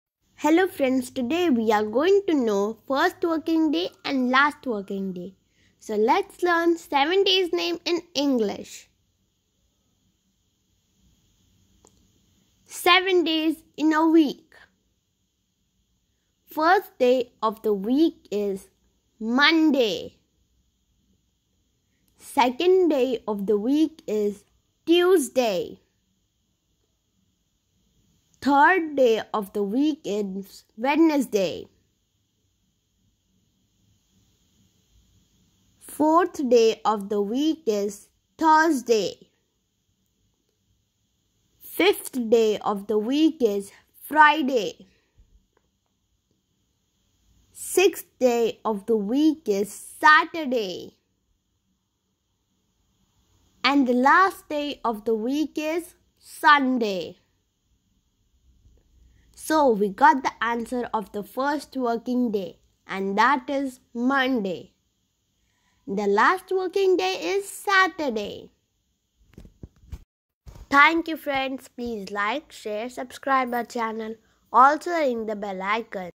7 day week name Days Of The Week | Sunday Monday Ki Spelling | Sunday To Saturday Spelling Writing
Days Of The Week | Sunday Monday Ki Spelling | Sunday To Saturday Spelling Writing |
First working day ?
Last working day ?
Hello Friends,
In this video we will learn how to write Sunday to Saturday with my beautiful and print ✍️ handwriting.

lot of ❤️❤️❤️

Last Working DAys ?
My top  video link is